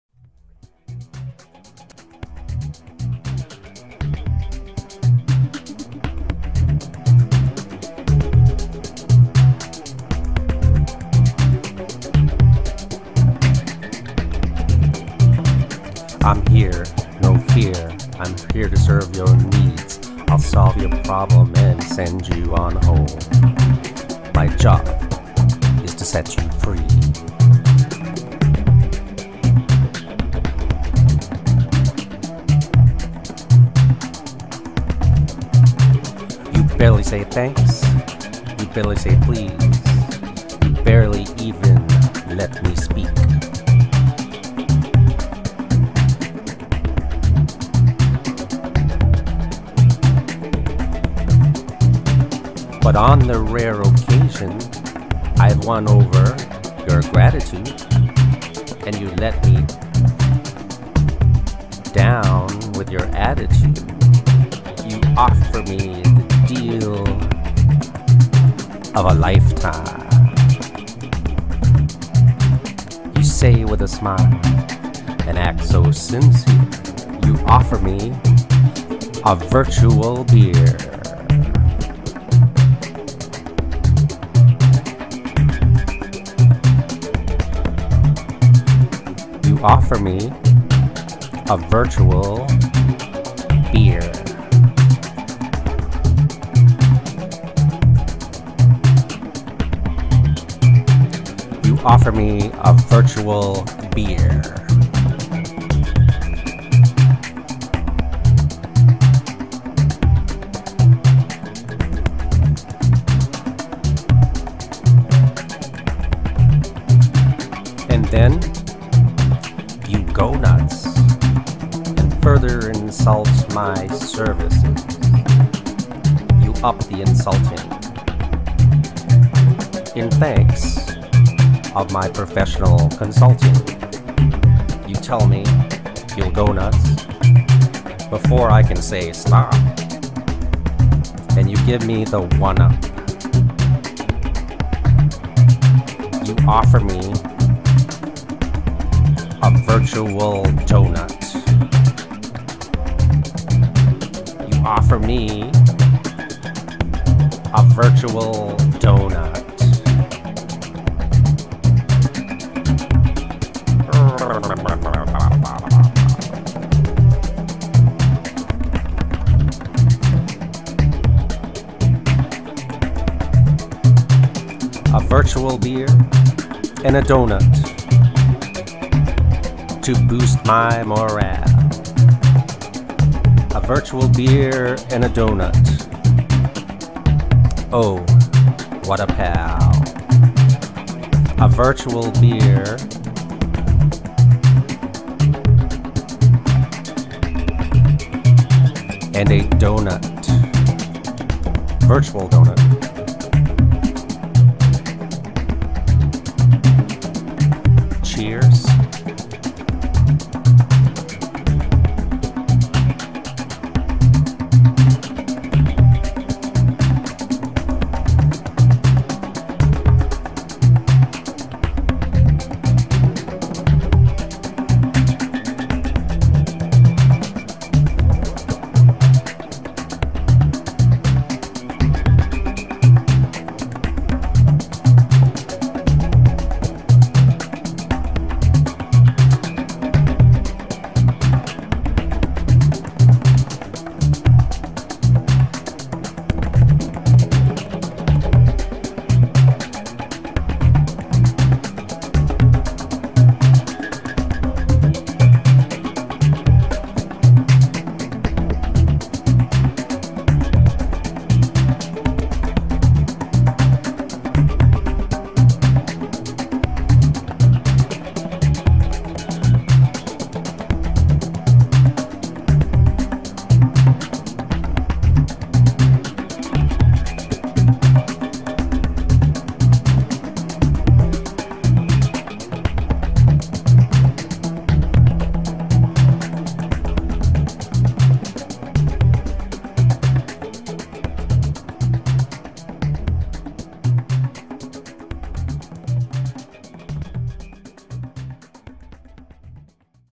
Original music by Kevin Ronkko
A day in the life in customer service.